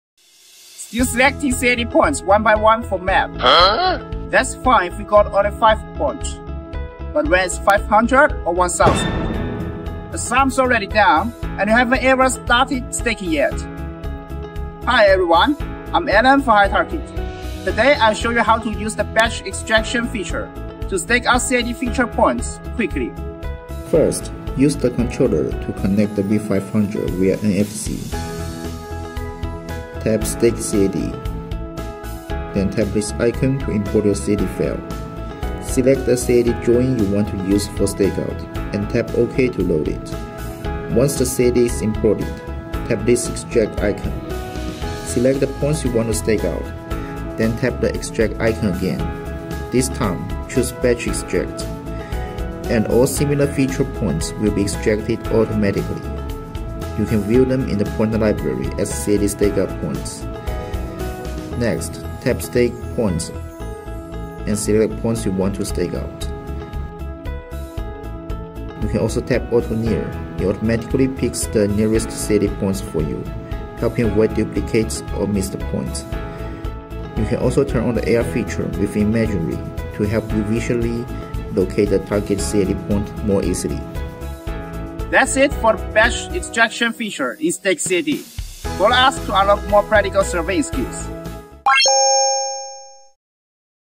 Still picking CAD points one by one?hitarget
When you’ve got 500 or 1000 points to stake, manual selection just won’t cut it. Try Batch Extract — extract 1000 points in minutes and stake them out fast.

YOUTUBE OR FACEBOOK:
Manually picking CAD points? That’s fine for 5 — but not for 500 or 1000.

In this video, Alan shows you how to use the Batch Extract feature to instantly select all CAD feature points and start staking them out in just a few taps.
From NFC connection to Auto Near selection and AR guidance, this is how modern surveying gets done — faster and smarter.

✅ Ideal for land surveyors working with large CAD drawings
✅ Boost your efficiency on construction or road layout projects